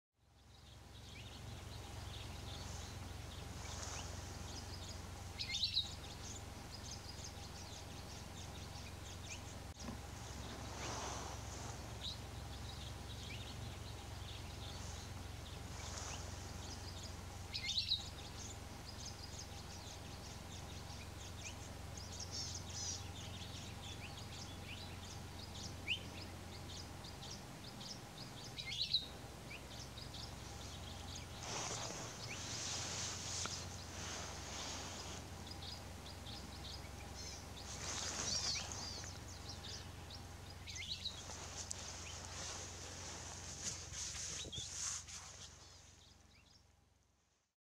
Palila, one of the rarest honeycreeper songbirds of Hawai'i (Big Island)
Palila group resting in a tree on 20 July 2015 at 729 am